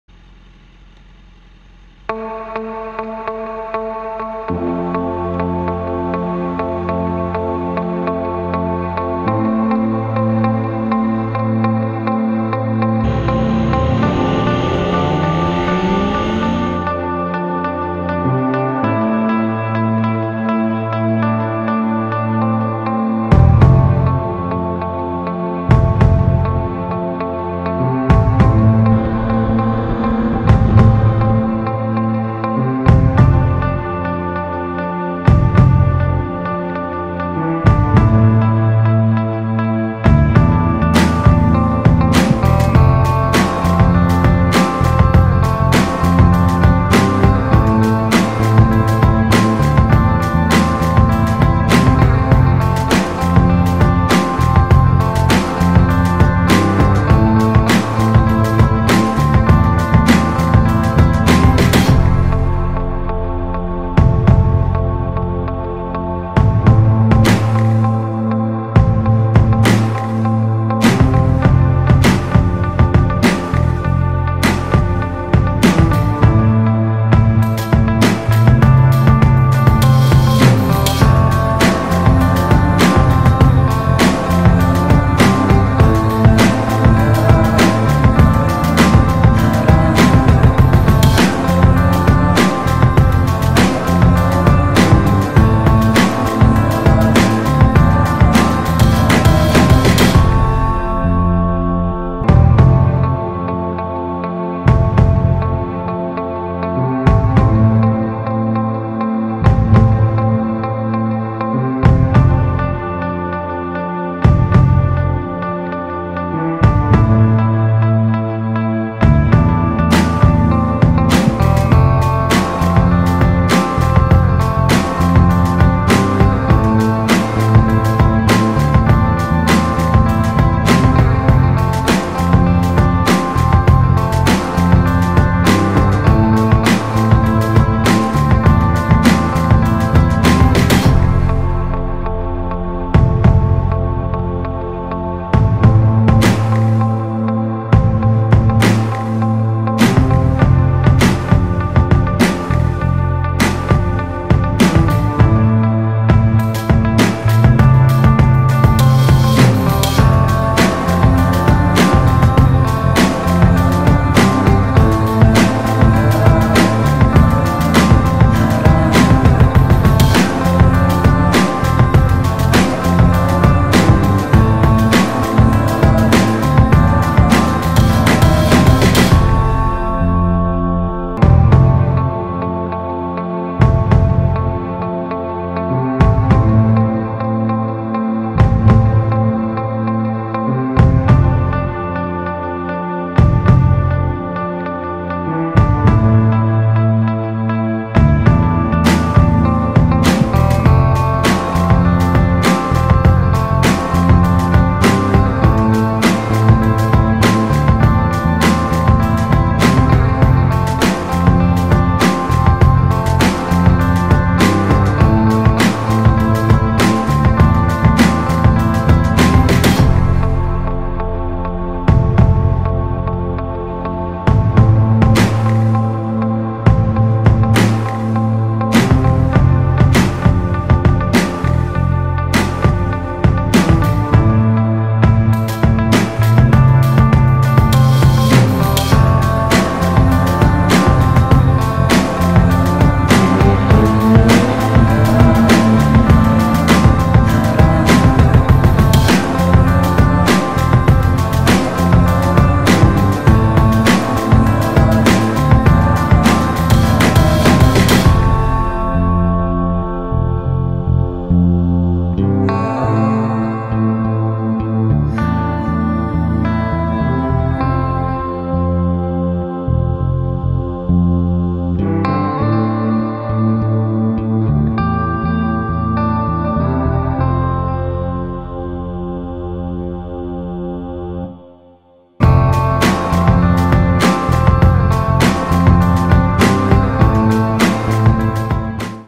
BMW HP4, Buell XB9, & Ducati Streetfighter 848 Riding the Backside of Angeles Crest Hwy
BMW HP4, Buell XB9, & Ducati Streetfighter 848. Riding the backside of Angeles Crest Hwy with my buddies. This is the last video I shot riding my HP4.